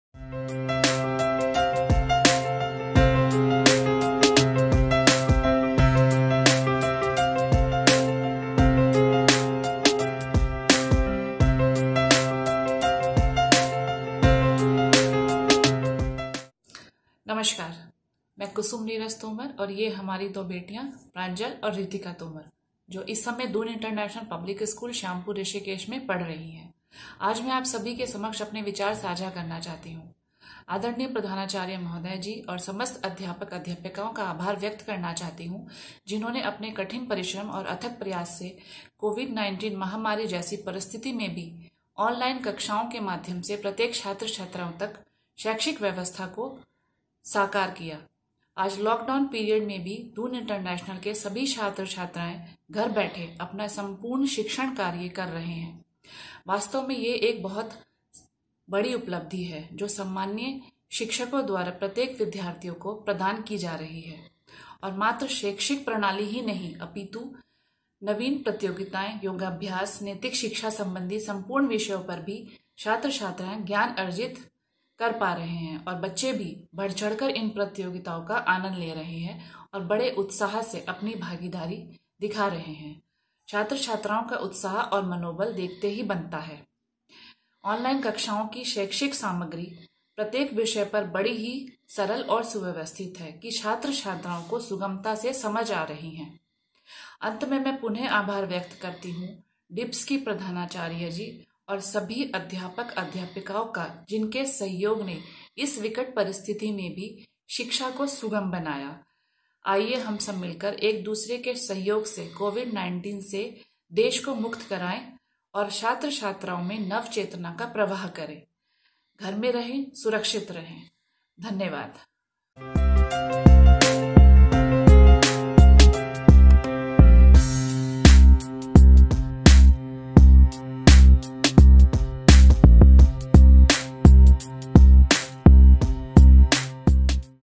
Parent's Testimonial and Review!!! Let's hear our parents!!!
“We all need people who will give us feedback. That’s how we improve.”
– Bill Gates

It is really important for us to ensure that we obtain Parent feedback regularly.
Parent's feedback allows us to see where our strengths lie and also the areas in which we need to improve.
In this order,here we are with our first feedback video featuring Mrs. KuSum N Tomar ,Mother of Pranjal_Tomar (Grade V) & Ritika_Tomar (Grade IV) .
We would like to thank Mrs.Tomar for her valuable feedback and for always being so supportive.

DIPs Rishikesh will now keep sharing the valuable feedback given by parents of our students.